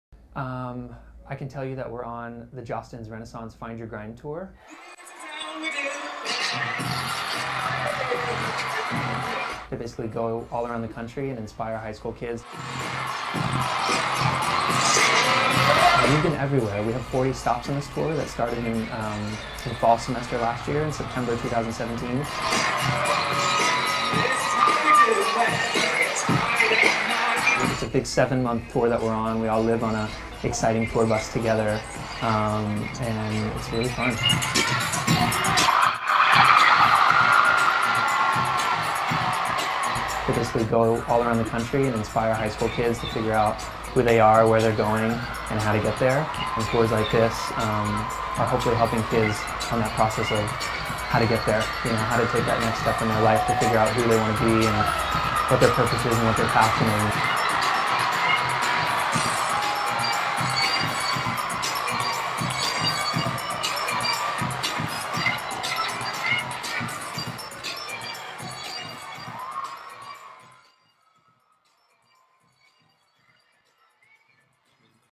Renaissance Music Tour comes to Bakersfield High School
Renaissance Music Tour comes to Bakersfield High School to talk to students about hopes and dreams.